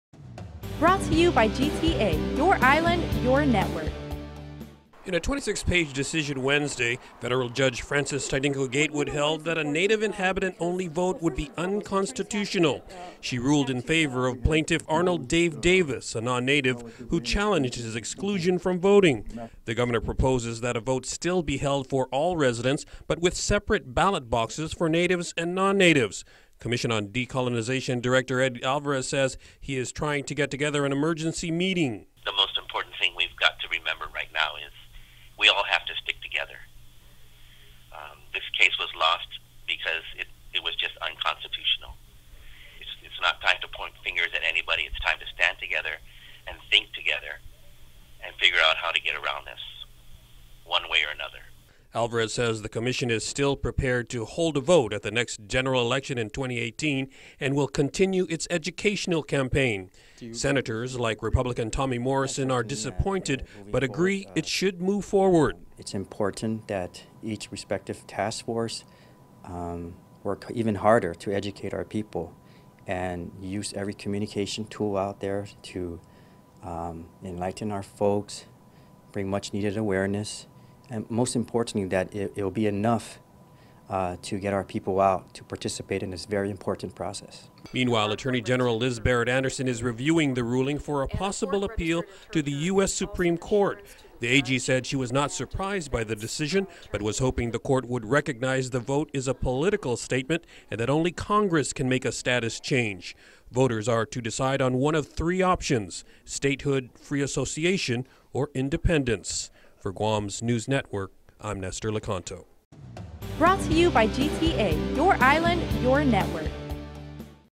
Leaders maintain commitment towards self-determination
Governor Eddie Calvo says he is deeply disappointed by the District Court of Guam's ruling in the plebiscite case, but still vows to move forward with a long-delayed vote on Guam's political status.